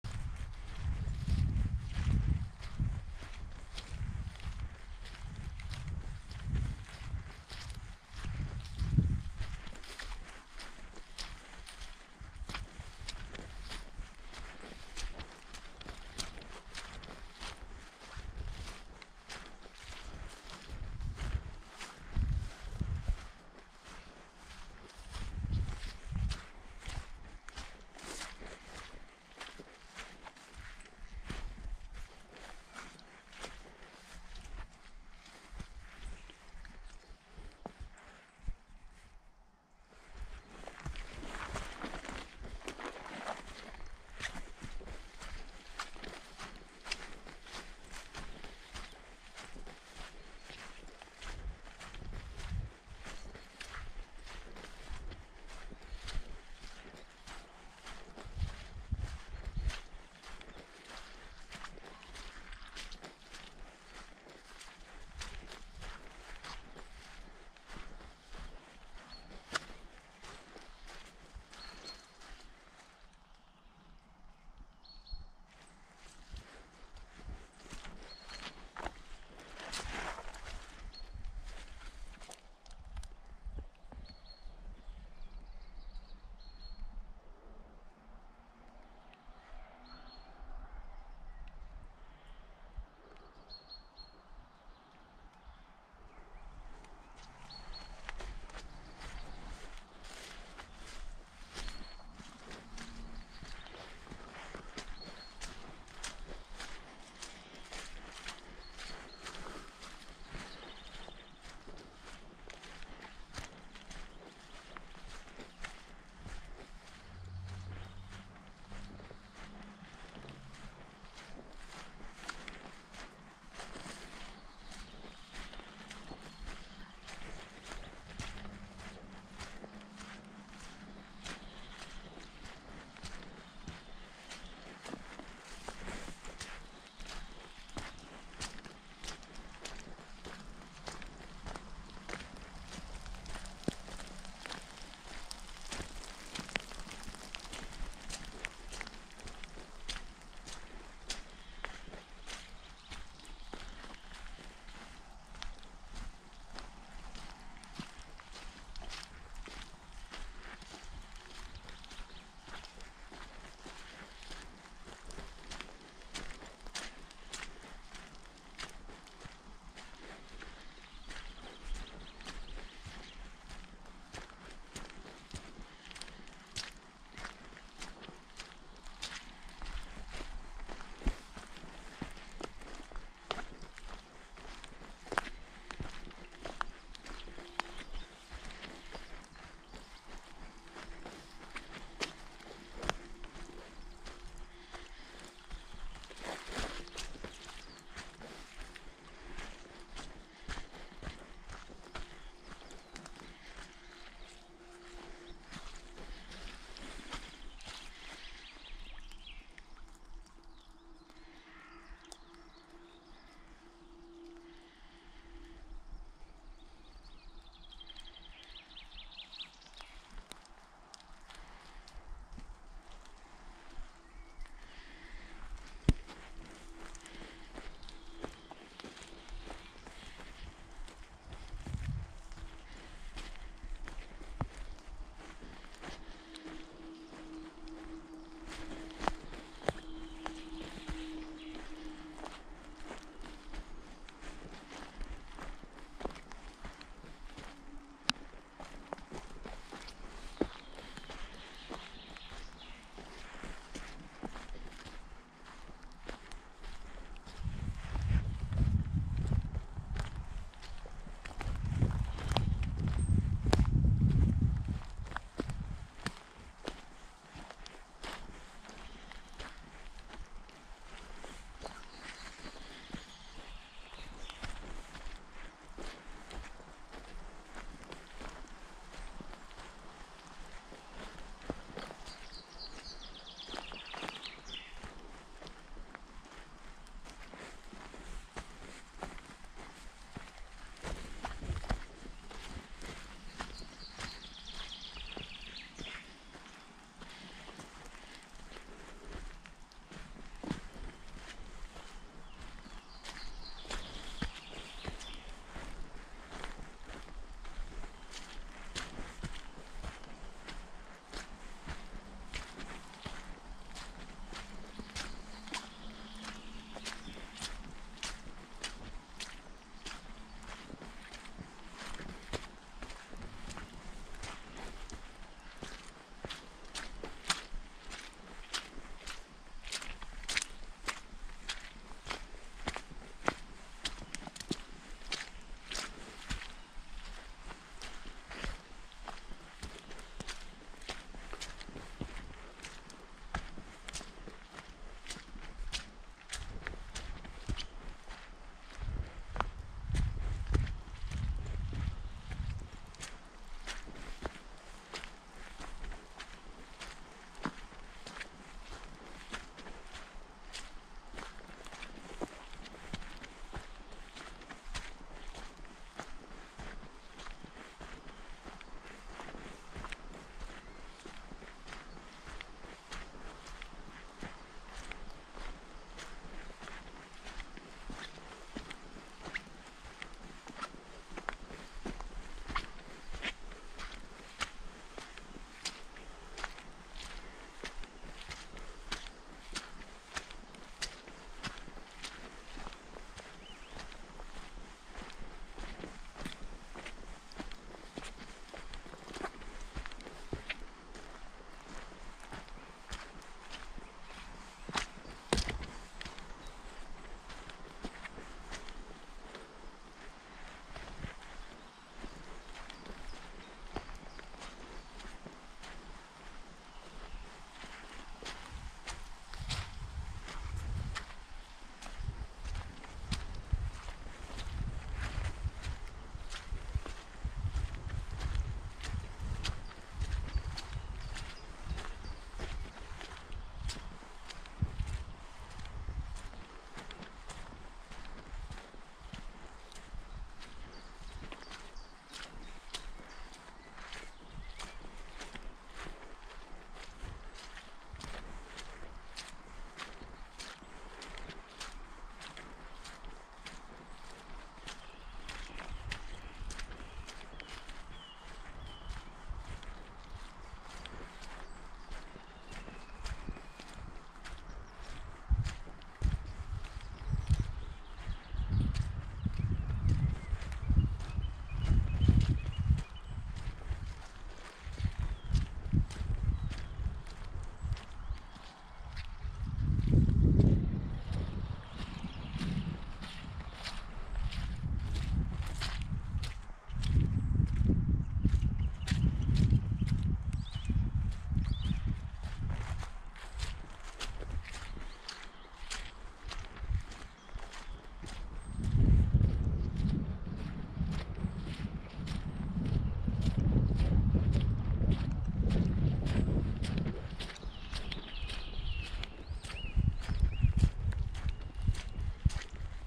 Harwood Dale: Muddy Puddle Trudge
A trudge through muddy puddles, in and out of trees, in light rain.